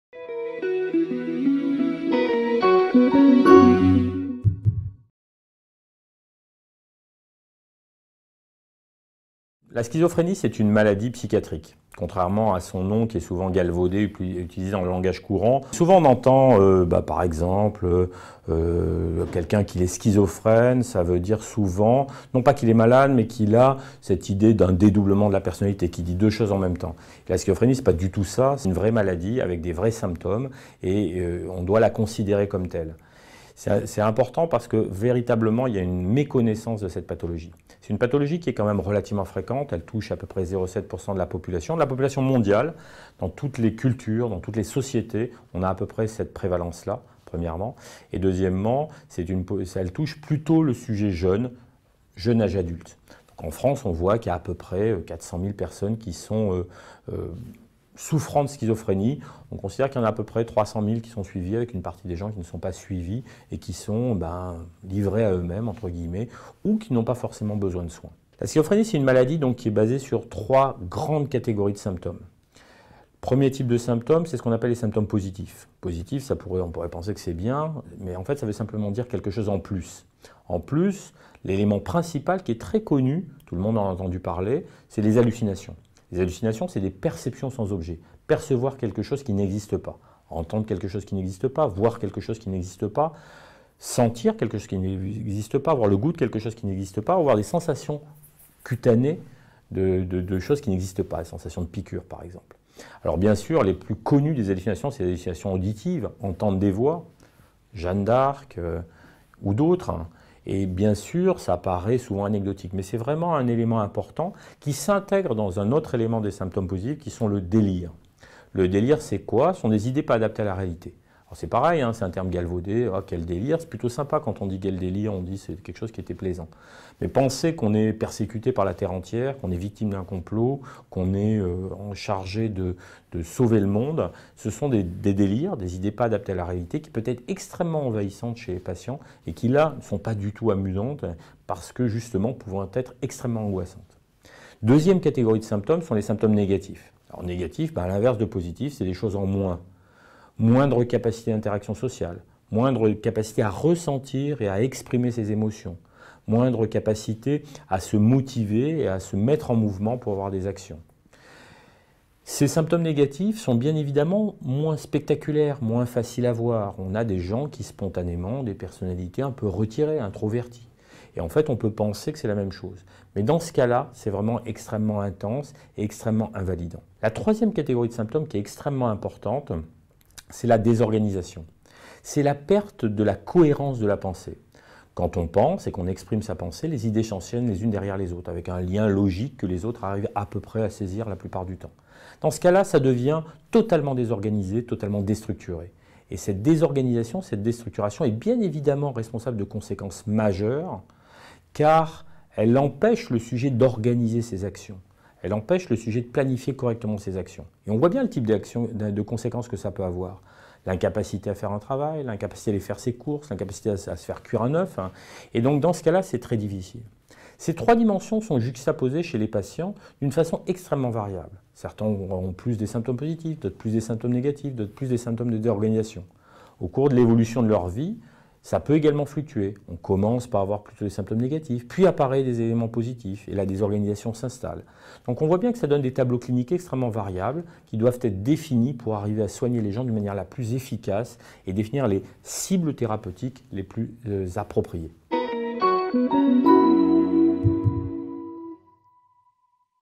Qu’est-ce que la schizophrénie ? | Do You Spoc
Employé à tort et à travers, le terme de « schizophrénie » est très souvent galvaudé. Le Professeur Pierre-Michel Llorca, spécialiste mondial de cette maladie, nous présente clairement en quoi elle consiste et quels sont ses symptômes.

Si vous pensez que la schizophrénie correspond à un dédoublement de la personnalité ou à un simple “délire”, vous faites fausse route ! La schizophrénie est une maladie plus complexe qu’on ne l’imagine. Décrite il y a 100 ans, elle reste encore l’une des maladies psychiatriques les plus mystérieuses, et les recherches dans ce domaine se poursuivent activement.
Autre phénomène méconnu : en plus des souffrances qu’elle inflige aux malades et à leur entourage, la schizophrénie s’avère une des maladies les plus coûteuses pour la société - son coût est en effet évalué à près de 15 milliards d’euros en France. Une meilleure connaissance de la maladie de la part du grand public est donc nécessaire pour mieux appréhender ce mal et accompagner les malades.